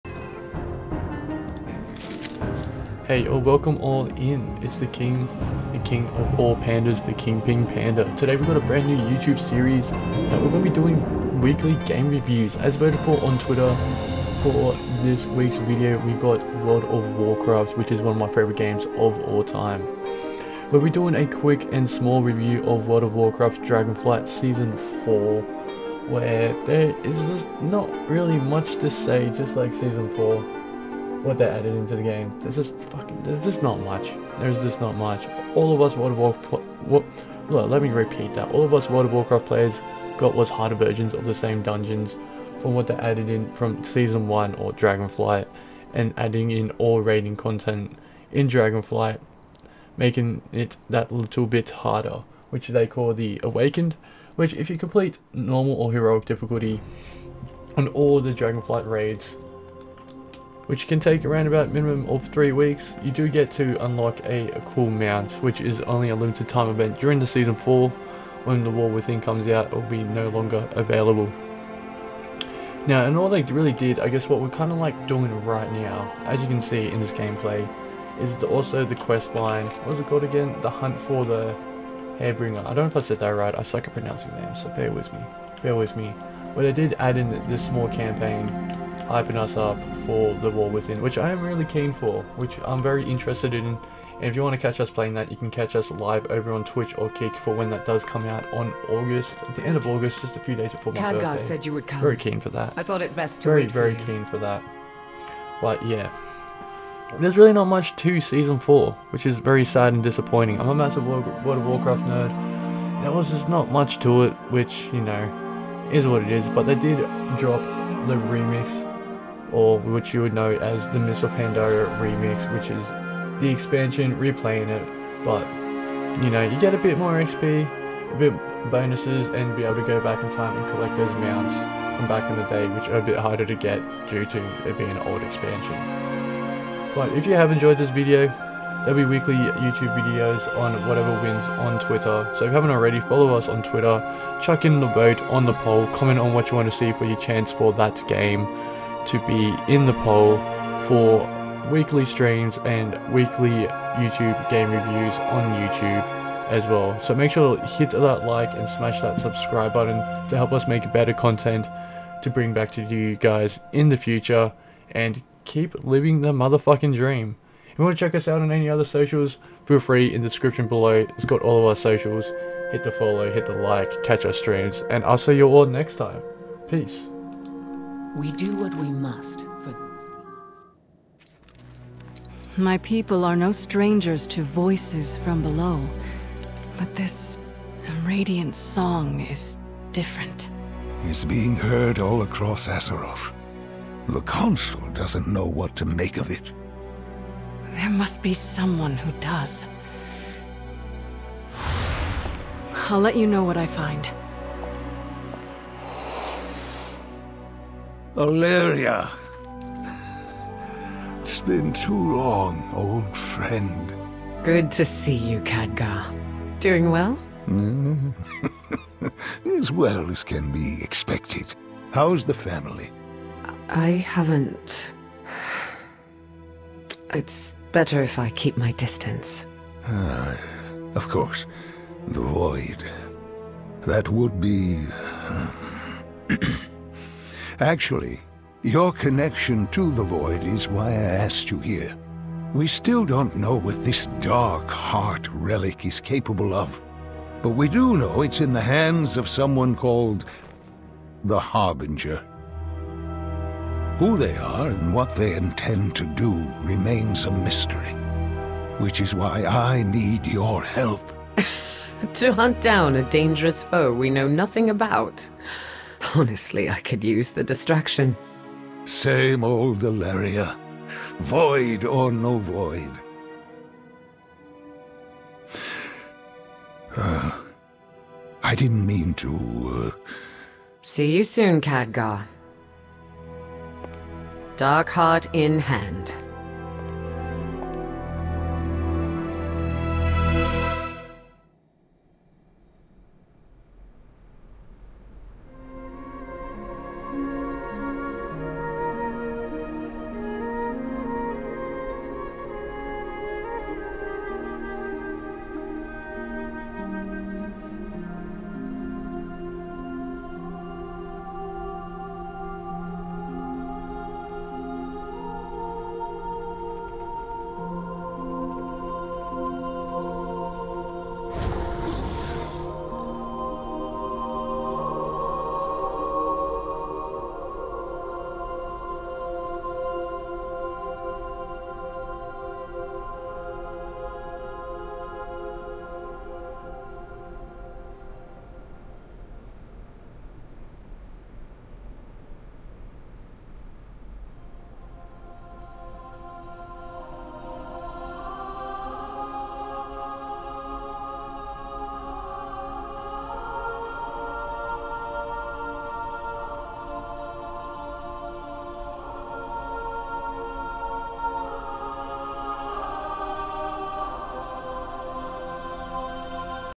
WoW DF Season 4 Game Review.
Explore the latest in World of Warcraft with our review of Dragonflight Season 4! Dive into new dungeons, epic battles, and exciting features that redefine gameplay in Azeroth. Join us as we break down what makes this season a must-play for WoW enthusiasts!